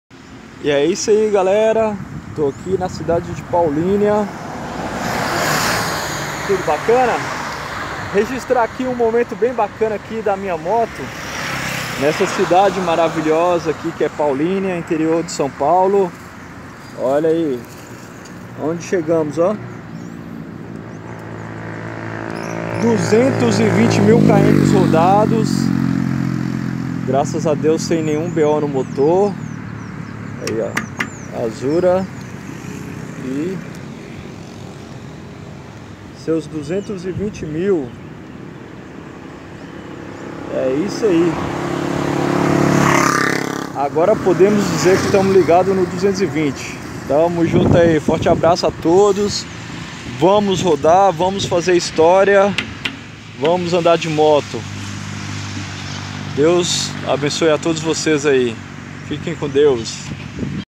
220 Mil Kms sem fazer o Motor - Fazer 250 220K
Marca expressiva,220 mil kms Rodados sem fazer Motor,Empreagem etc...
Um verdadeiro Tanque de 2 rodas...